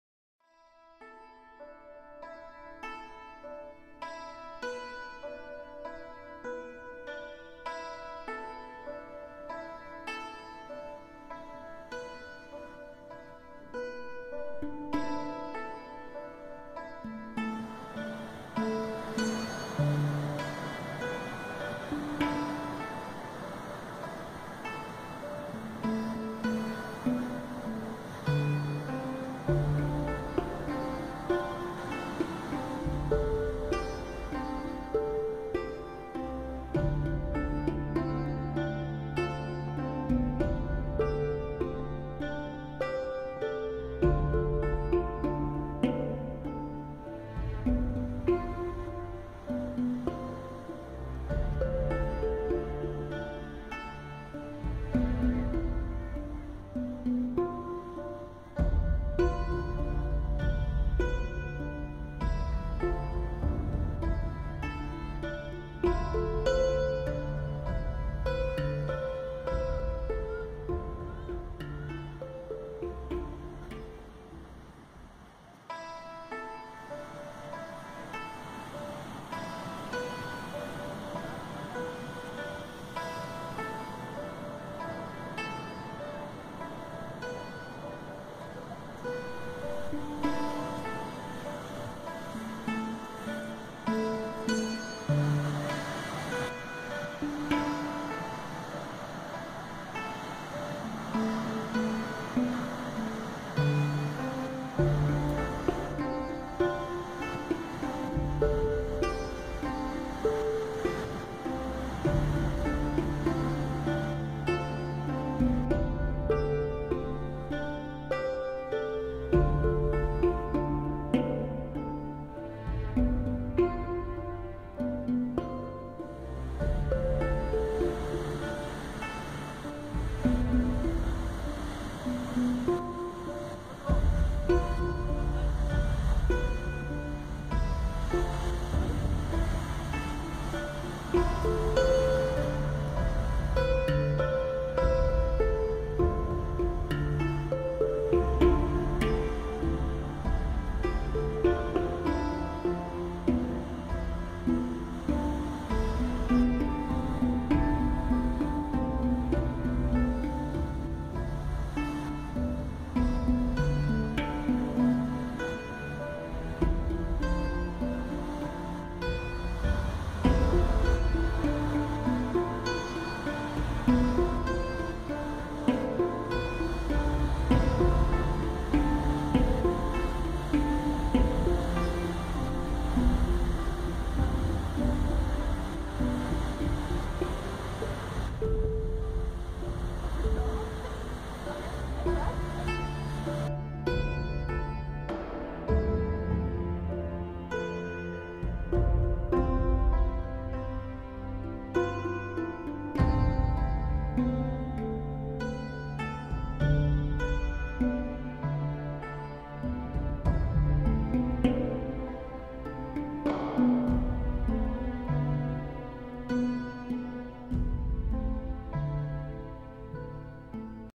Amma Traditional Japanese Massage Therapy - Natura Cabana
Filmed at Natura Cabana in the Dominican Republic
Allez au-delà du rêve et expérimentez le massage Amma.  Vous vous sentirez bercés par la brise et le bruit des vagues que vous soyez au Québec, au studio de Sylvie Girard ou à l'Écochalet du Camp Comfort ou encore en République Dominicaine, à Cabarete dans les Caraïbes où ces images ont été saisies par Tom Axtell.